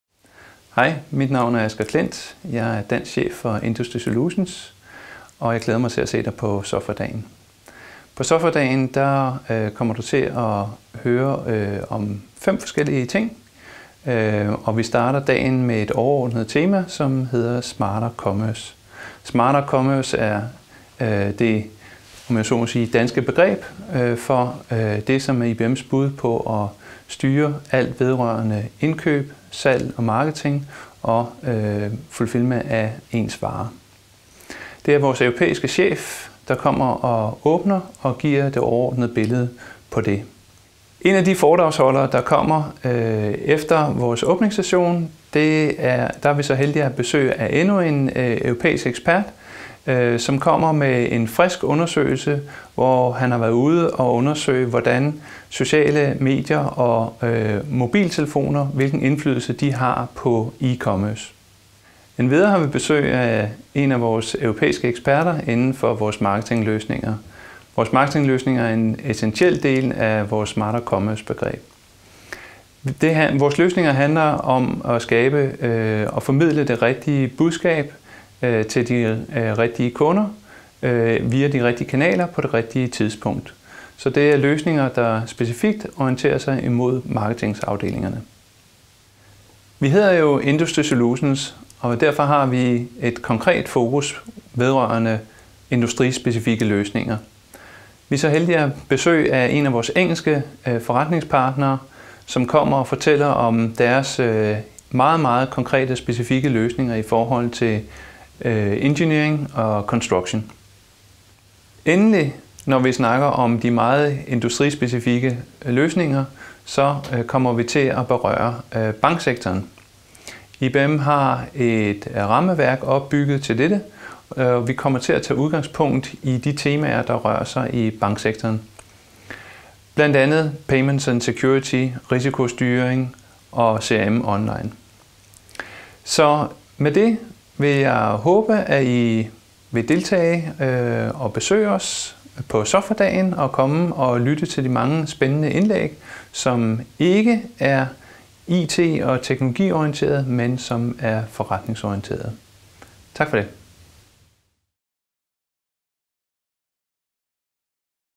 Smarter Commerce
Asger Klindt, Country Manager i IBM Industry Solutions, giver en introduktion til, hvad du kan høre på IBM Smarter Business 2011.